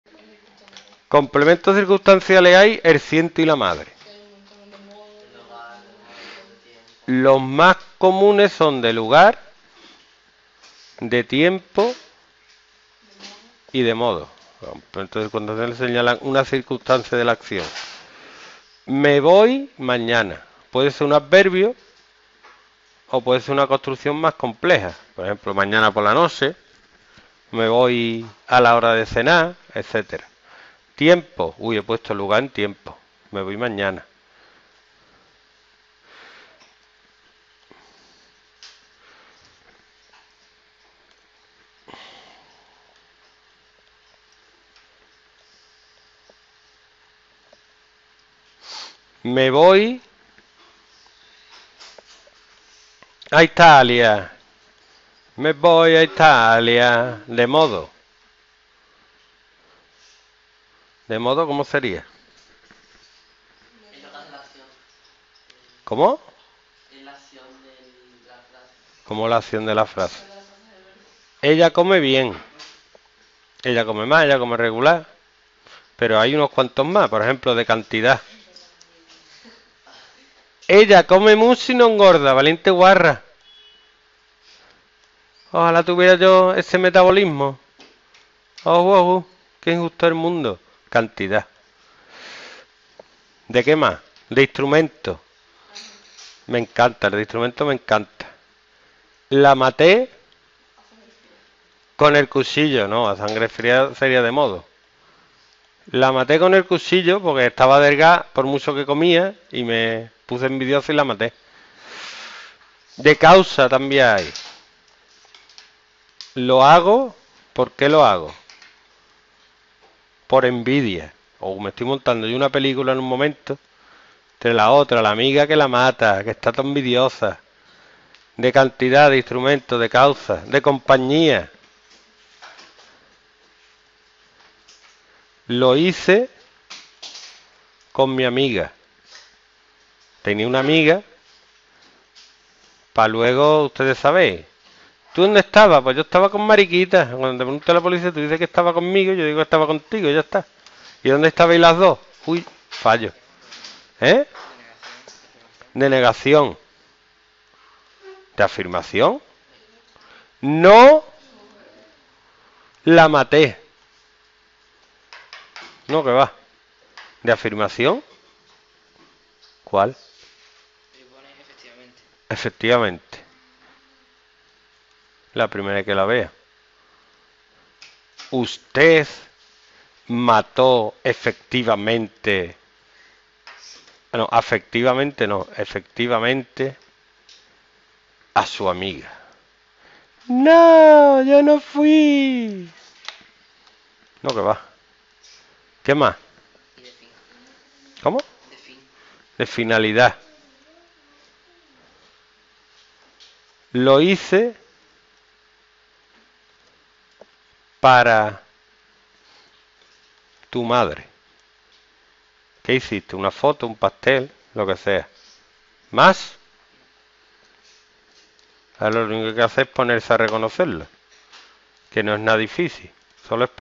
Complementos circunstanciales Lengua 2º ESO Academia Usero Estepona
Los complementos circunstanciales permiten expresar las circunstancias en las que se produce la acción descrita por el verbo. Los más habituales son: